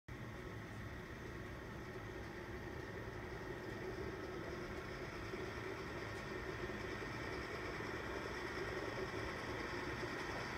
Used Cat 140G Motor Grader For Sale
1985 Cat 140G Motor Grader For Sale
Serial #: 072V08376
Hours: 9999
Fully operational, City trade in good working condition.